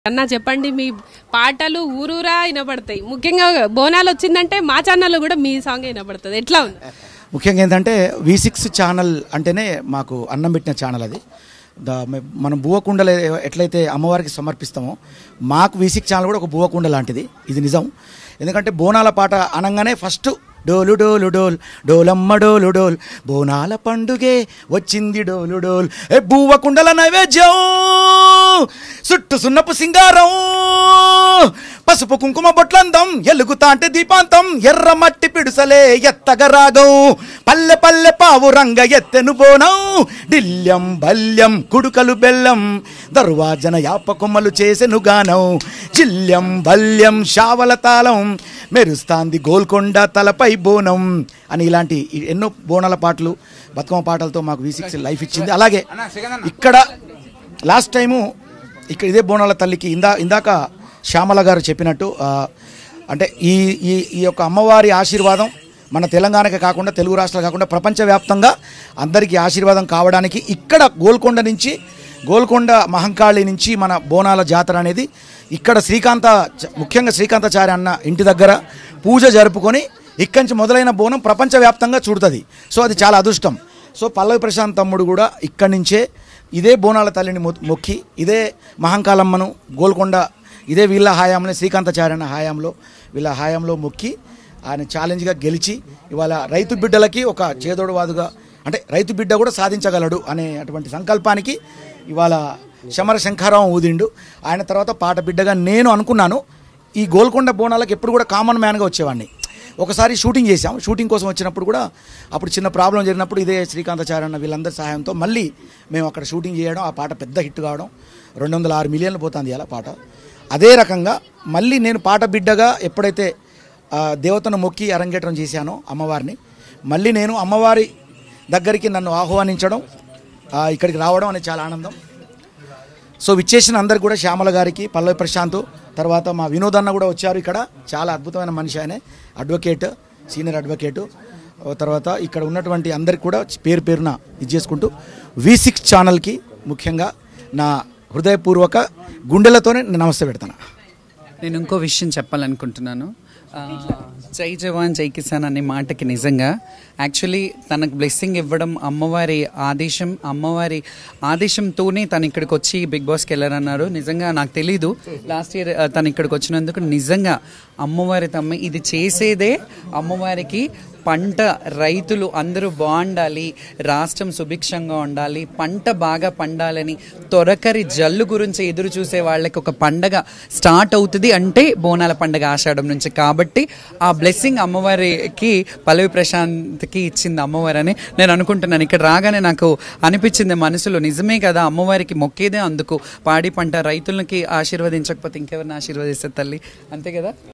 F2F with Singer Bhole Shavali | Golconda Bonalu 2024 | V6 News
వెజ్ నాకెంత..?